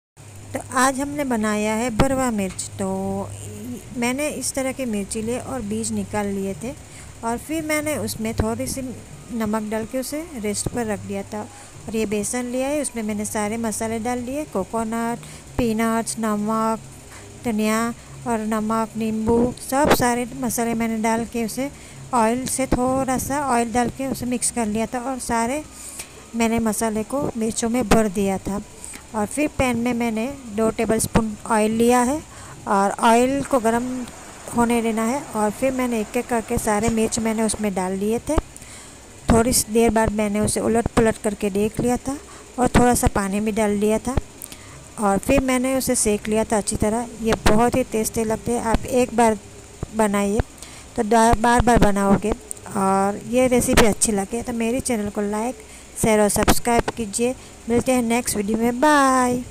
भरवा मिर्ची बनाना सिखे|Stuffed green chilli 😘😘
Ingredient:-
green chili
sesame seeds
ground nuts
coconut
besan
turmeric powder
garmmasala powder
lal mirch powder
dhaniya powder
salt
oil
sugar